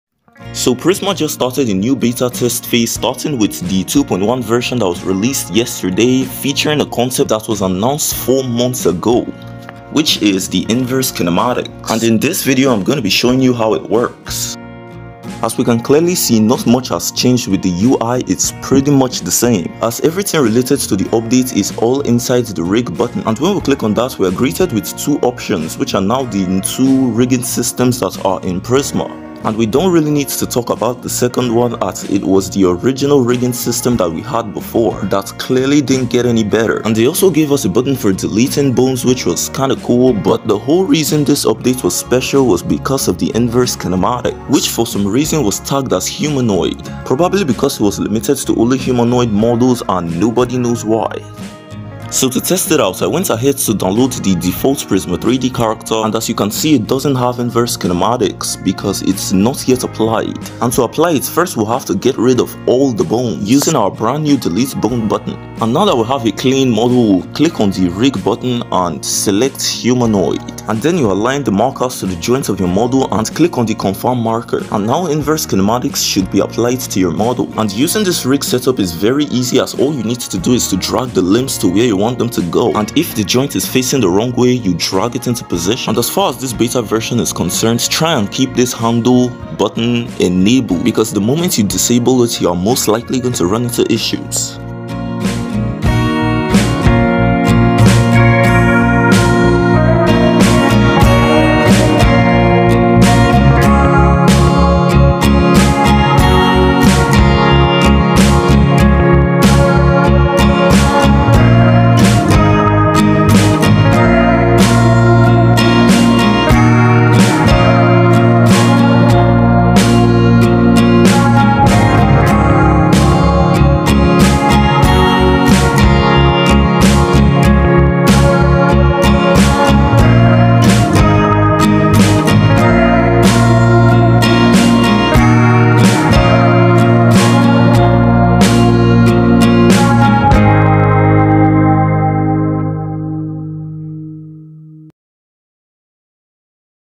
Mobile Animation Just Got Easier : Prisma 3D 2.1
In this video I'll be breaking down the newest Prisma 3D update with the special animation/rigging tool inverse Kinematics..
as animating in Prisma just took a drastic turn now animating with your phone is far easier than it was before..